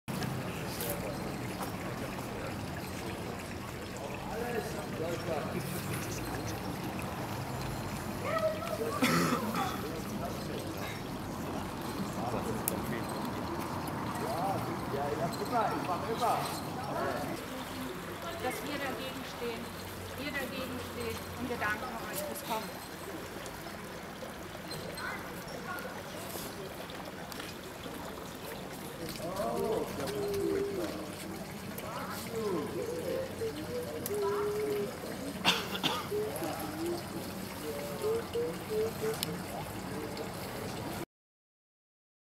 Neuburg: Mahnwache gegen rechten Terror
"Neuburg ist bunt": 50 Menschen haben in Neuburg an einer Mahnwache gegen rechten Terror teilgenommen.